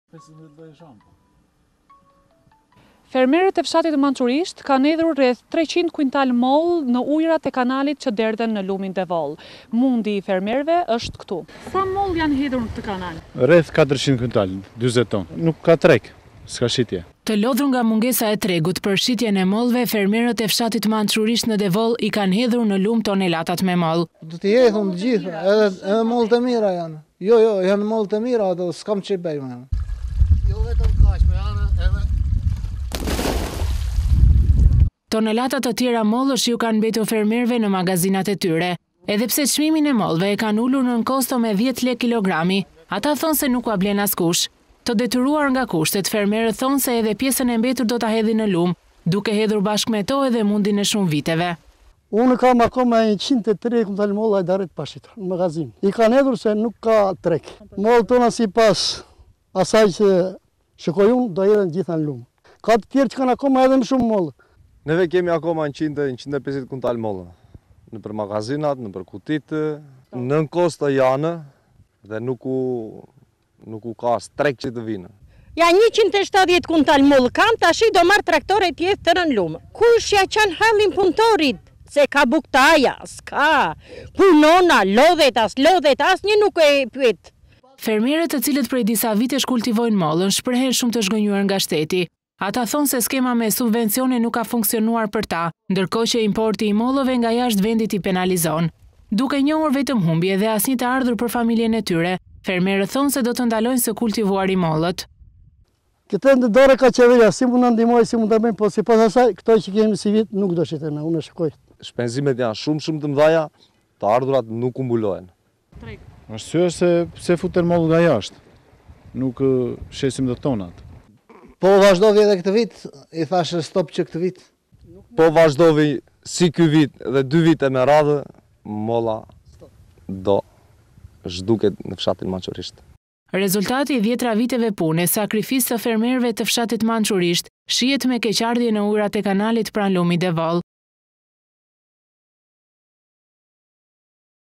Shihni nje lum mollesh sic nuk e keni pare asnjehere me pare, flasin fermeret
Shihni nje lum mollesh sic nuk e keni pare flasin fermeret